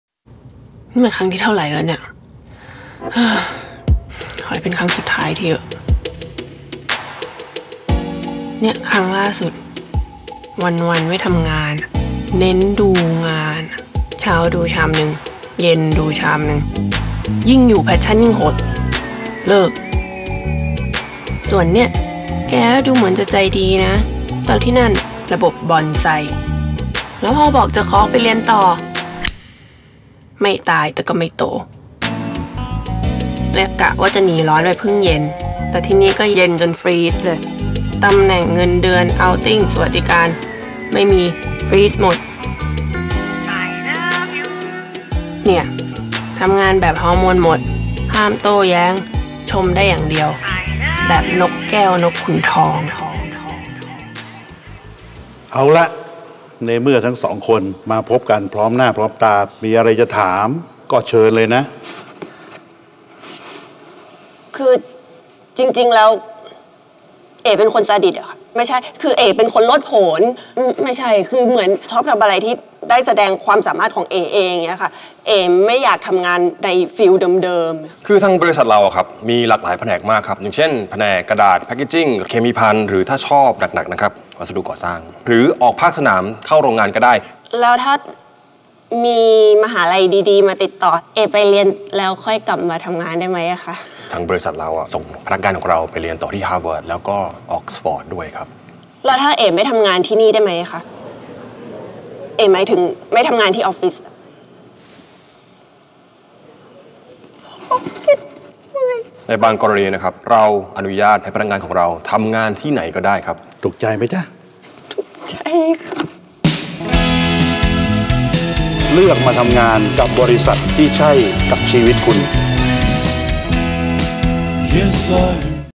Dream Workplace การแต่งงานครั้งสุดท้าย 2017 (Eng Sub) - SCG
เจ็บมากี่ครั้งแล้ว ถามตัวเองดูดี ๆ ว่าคนแบบไหน "ใช่" กับชีวิตคุณ

เปลี่ยนวิถีการทำงานเดิมๆ ที่ SCG มองหาคนที่ต้องการแสดงความสามารถของตนเองในการทำงาน ที่ตอบโจทย์ความต้องการของคนรุ่นใหม่ มีการจัดโปรแกรมเรียนต่อต่างประเทศ และอนุญาตให้พนักงานสามารถทำงานนอกบริษัทได้ตามใจชอบ
เลือกมาทำงานกับบริษัทที่ใช่กับชีวิตคุณ

เข้ามาเป็นส่วนหนึ่งของครอบครัว SCG l

ติดตาม เอสซีจี เพิ่มเติมได้ที่